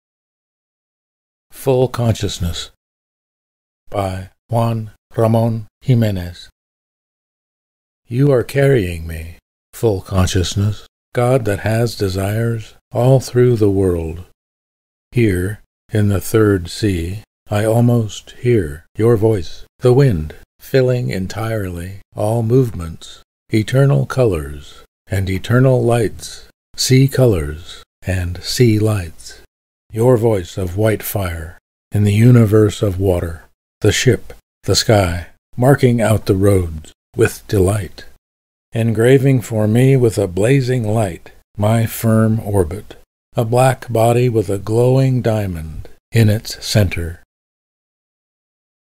"Full Consciousness," by Juan Ramon Jimenez
Poem, translated by Robert Bly. Jimenez (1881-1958) was born in Moguer, Spain, and won the Nobel Prize in Literature in 1956. Images from 8929, Free-Photos, Limat MDARIT, Volens Nolens, Manfred Zajar, Pexels, MartinStr, Frank Winkler, and Danehrr22 on Pixabay, used by permission: thank you.